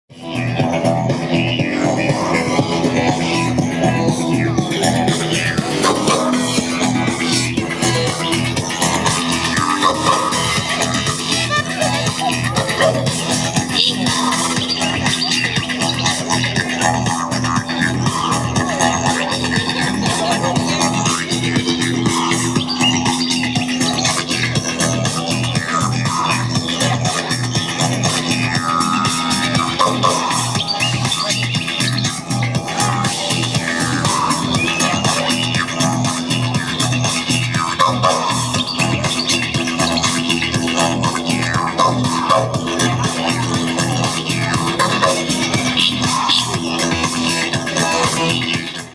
groovy Didgeridoo
A wee bit too short a video for the amount of natural talent this guy has...
Nonetheless, I had to share.